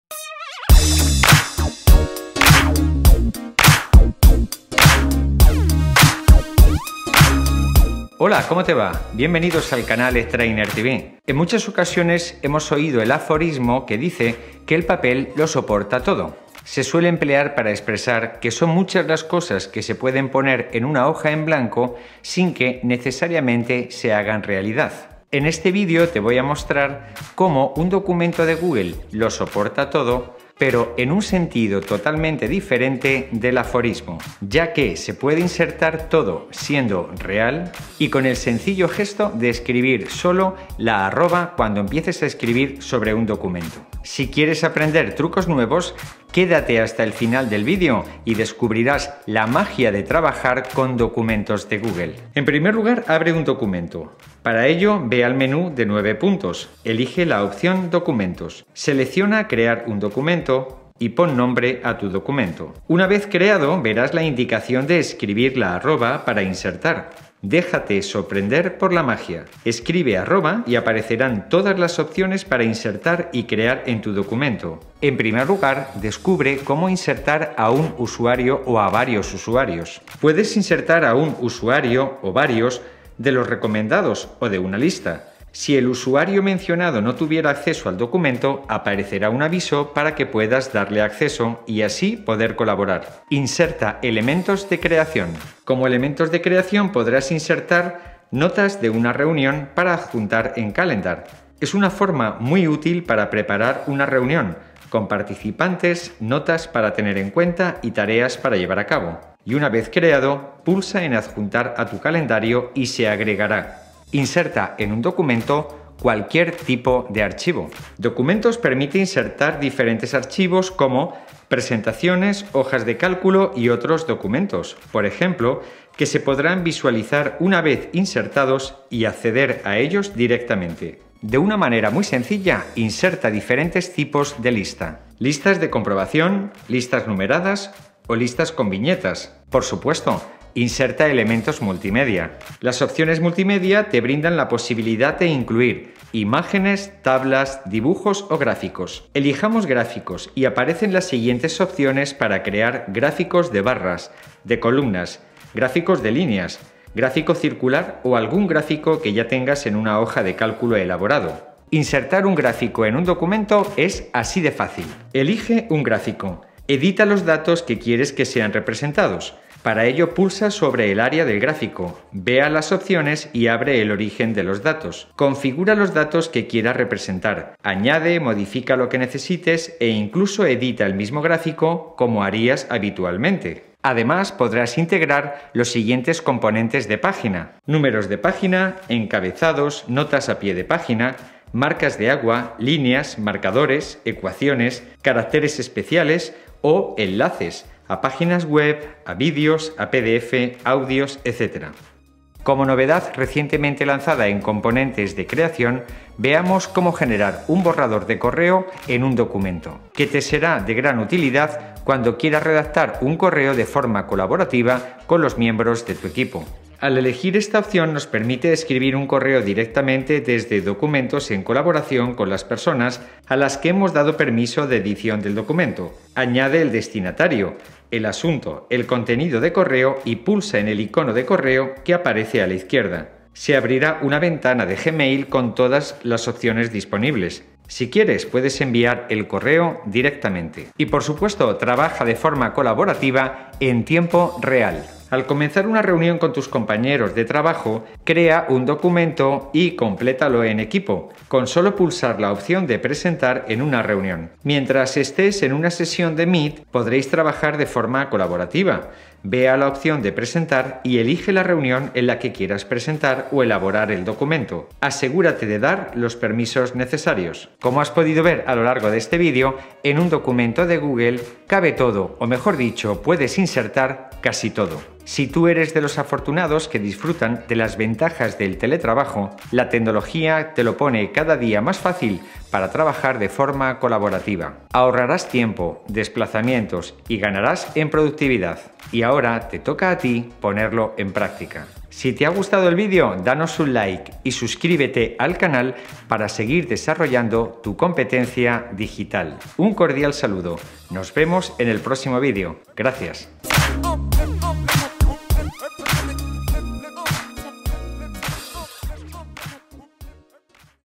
Documentos de Google: Escribe @ para insertar
¿Sabías que con solo escribir @ en Google Docs puedes insertar personas, archivos, gráficos y más sin complicaciones? Descubre cómo esta función mejora la productividad y la colaboración en tiempo real.

🎯 En este video aprenderás:
✅ Cómo insertar usuarios y gestionar accesos con @.
✅ Cómo agregar listas, gráficos y archivos sin salir del documento.
✅ Cómo redactar correos desde Google Docs y enviarlos con Gmail.
✅ Cómo presentar y colaborar en documentos dentro de Google Meet.
✅ Trucos avanzados para mejorar tu flujo de trabajo con @.

💡 ¿Ya usas @ en Google Docs? Cuéntanos en los comentarios.

⏱ 00:00 – 🎬 Inicio: Potencia Google Docs con la Función @
🔍 00:52 – ✨ Cómo Usar @ para Insertar Elementos en un Documento
👥 01:14 – 🔗 Añadir Usuarios y Gestionar Permisos con @
📝 01:33 – 🛠 Insertar Elementos de Creación (Notas, Tareas y Más)
📂 01:54 – 📎 Adjuntar Archivos Directamente en el Documento
📑 02:11 – ✅ Crear Listas de Tareas, Viñetas y Numeradas
🎨 02:19 – 🖼 Agregar Imágenes, Videos y Otros Elementos Multimedia
📊 02:20 – 📈 Insertar y Editar Gráficos en un Documento
📌 03:07 – 🏷 Añadir Encabezados, Páginas y Otros Elementos de Formato
📧 03:26 – 📤 Redactar y Enviar Correos desde Google Docs
🎥 04:00 – 📡 Presentar un Documento en una Reunión de Google Meet
📢 04:27 – 🎯 Sugerencias y Recomendaciones Finales

🔷Tecnología vs Libros: ¿Por qué NO debemos elegir? Tecnología vs Libros: ¿Por qué NO debemos elegir?